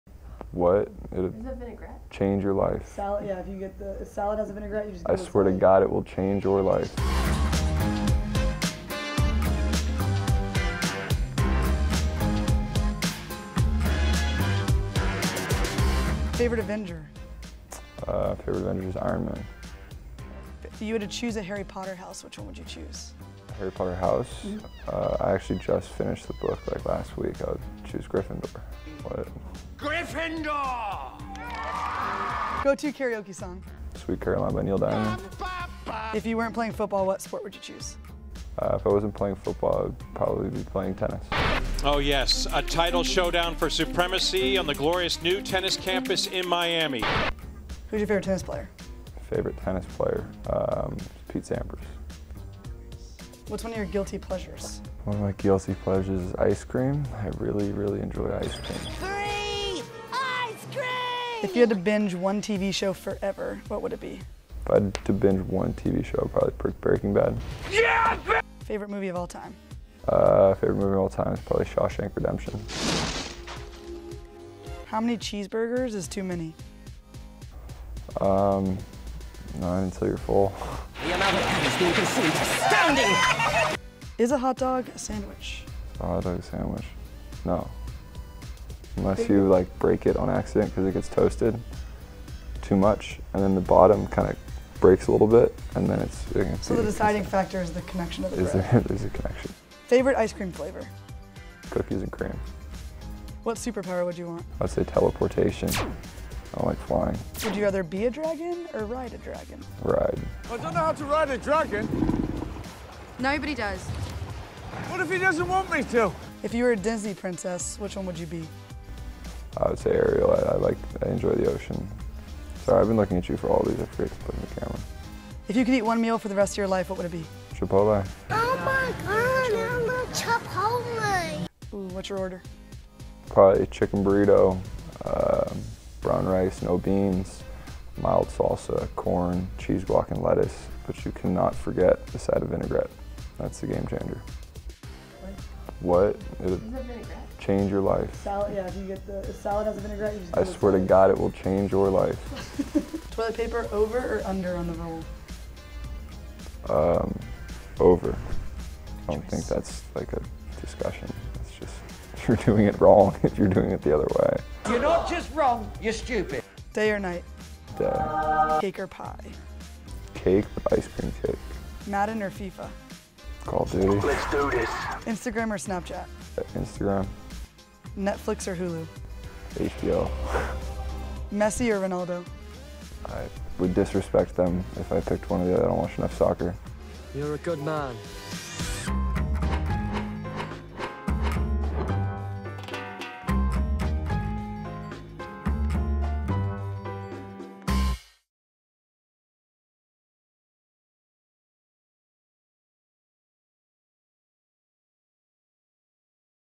Josh Rosen reveals his Chipotle hack - Q&A #9
Pro tip from Josh: never forget a side of vinaigrette with your Chipotle order 🌯

Check out the latest Q&A!